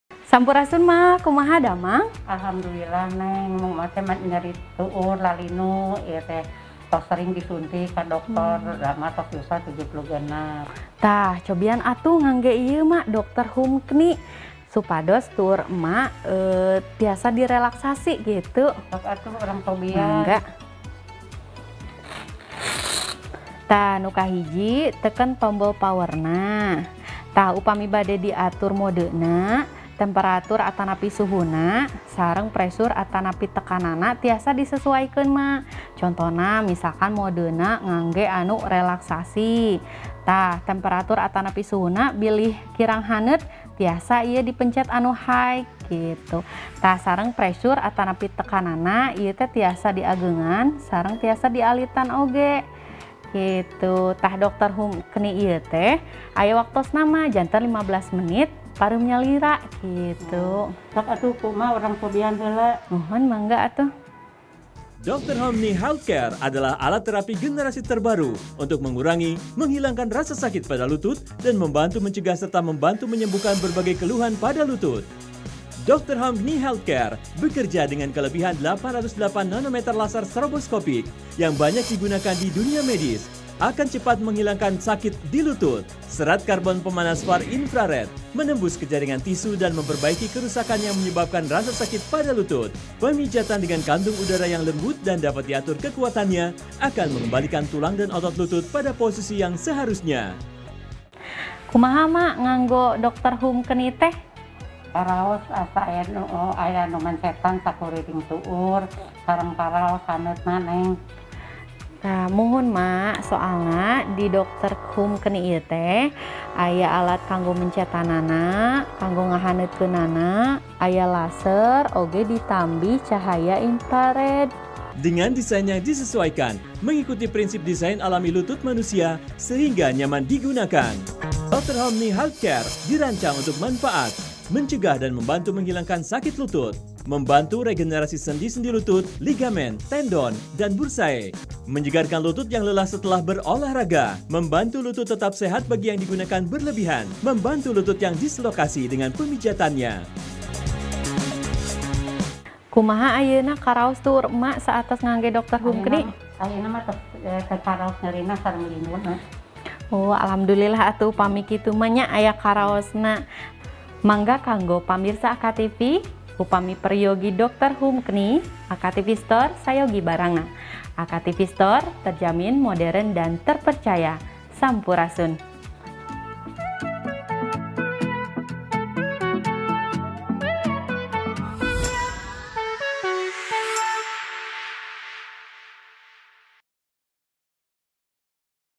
Terapi Lutut DR. HOOM Knee Health Care
Manfaat:
1.Melancarkan peredaran darah
2.Menghilangkan sakit/nyeri pada lutut
3.Mengembalikan posisi tulang & otot lutut pada posisi yang seharusnya
4.Membantu meregenerasi sendi-sendi pada lutut
5.Menyegarkan lutut setelah olahraga

DR. HOOM Knee Pain Therapy adalah alat terapi generasi terbaru untuk mengurangi/menghilangkan rasa sakit pada lutut & membantu mencegah serta membantu menyembuhkan berbagai keluhan pada lutut. Seperti lutut berbunyi pada waktu berjalan, lutut sakit waktu naik tangga/berjalan, lutut trauma karena jatuh/terbentur. Dan membantu regenerasi sendi lutut & membantu membuat rileks otot disekitar lutut.

Spesifikasi produk:
Rated Voltage: 220V
Rated Power: 12.5W
Dimention: 19.3x16.8x14.4 cm